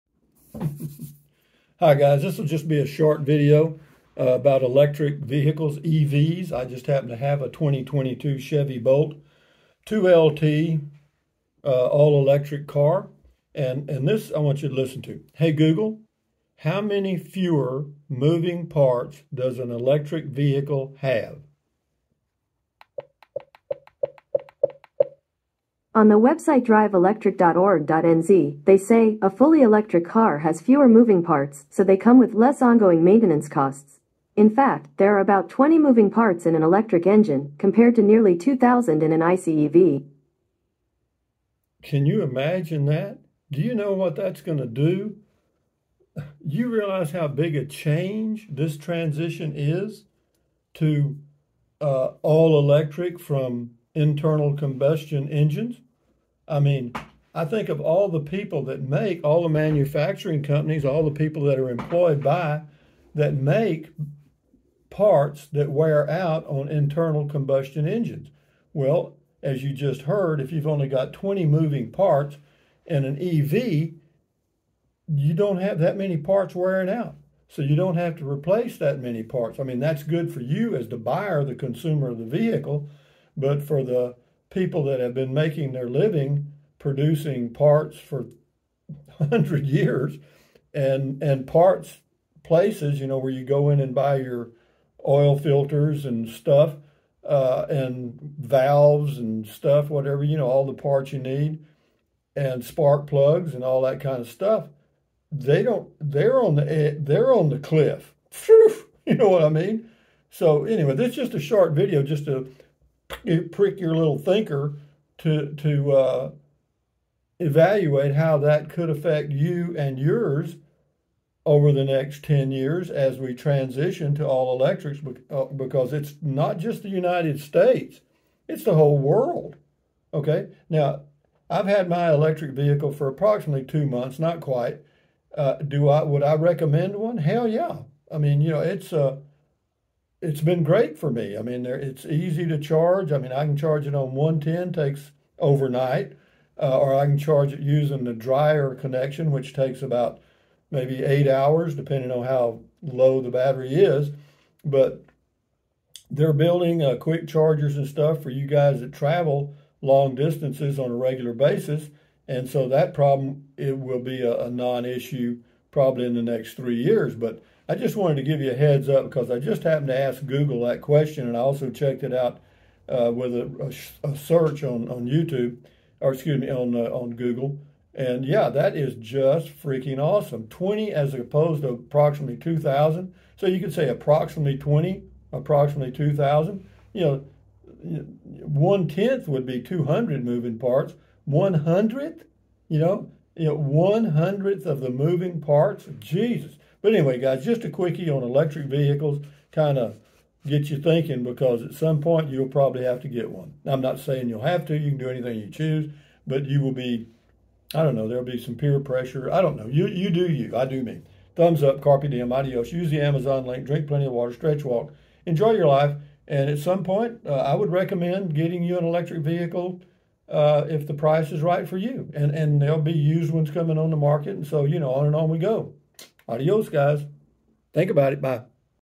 EV's...?
I own a 2022 Chevy Bolt 2LT...for me it's been a great car to have. Are there "FEWER MOVING PART" as compared to an INTERNAL COMBUSTION ENGINE...YEP!!!   thanks!!!